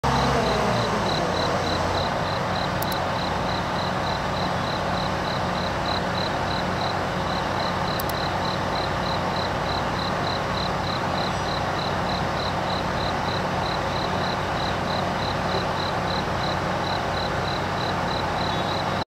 2015.5.13 台鐵 平溪線 更換鋼梁 十分 新舊鐵橋暫存場作業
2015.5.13 台鐵 平溪線 更換鋼梁 十分 新舊鐵橋暫存場作業